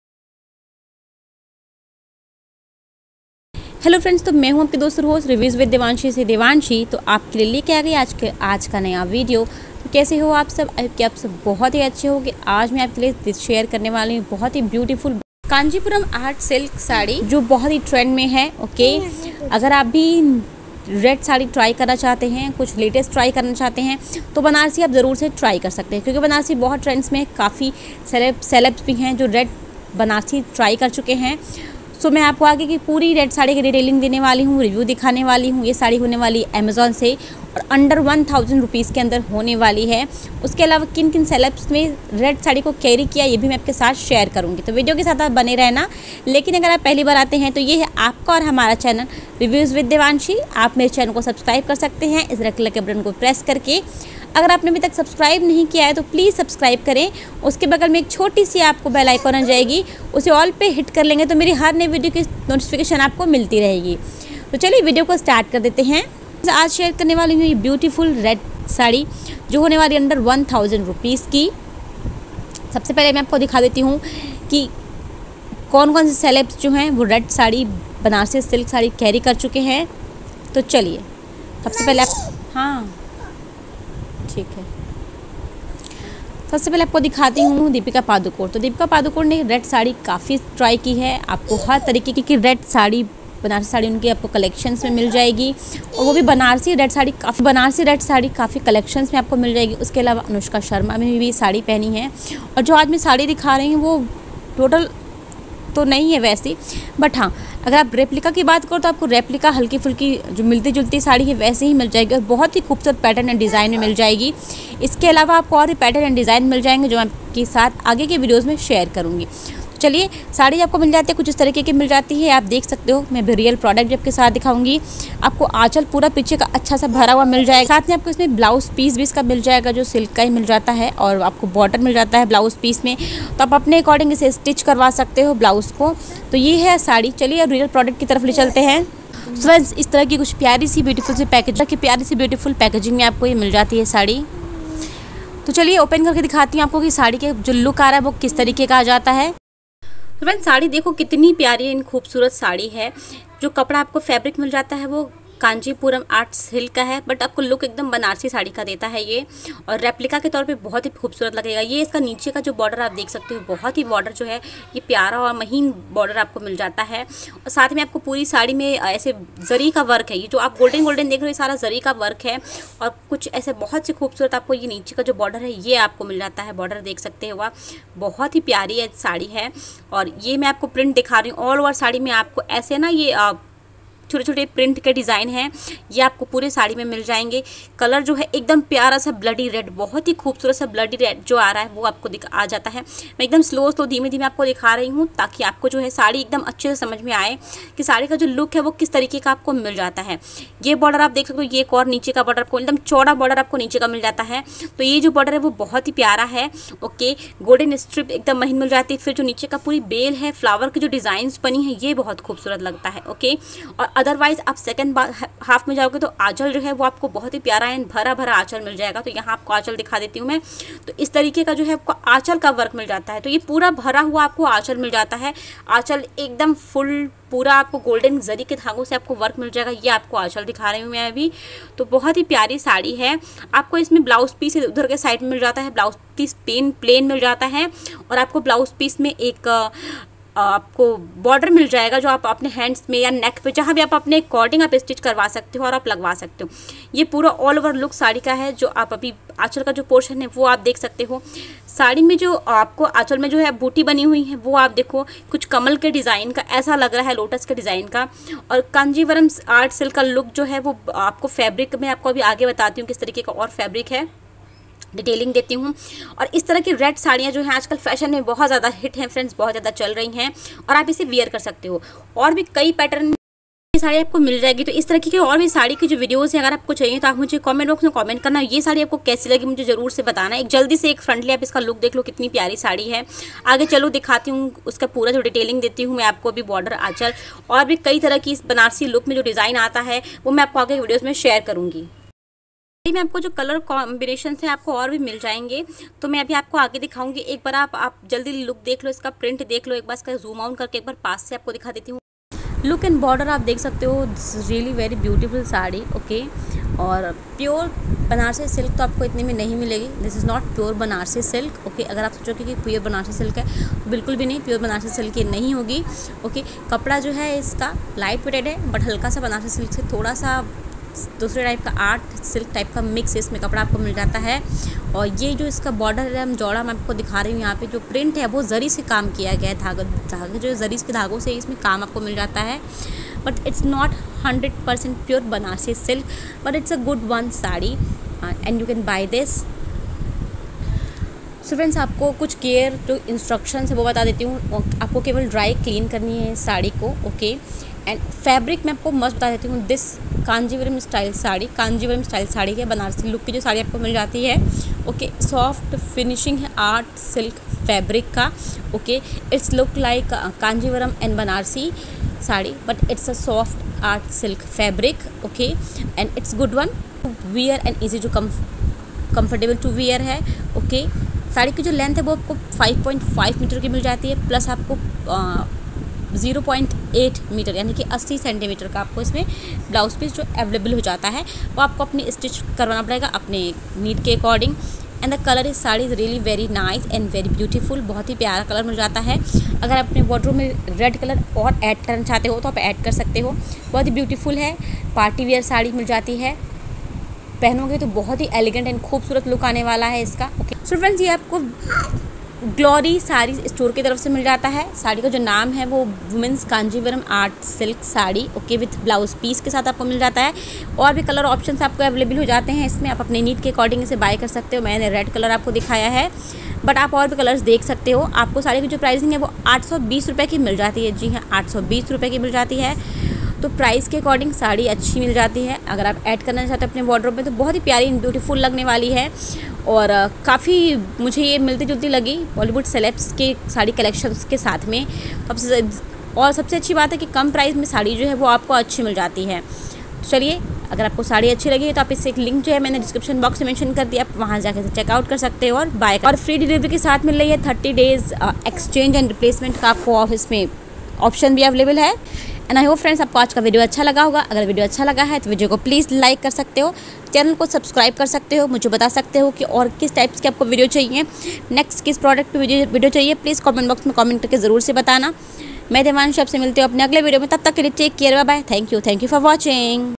Amazon Red Kanchipuram Art Silk saree Review/ Under 1000 Rs Saree
Hello friends. Amazon Red Kanchipuram Art Silk saree Inspired by Anushka Sharma Banarasi Red saree look share kar Rai hu. Video pasand aata hai to Like kare.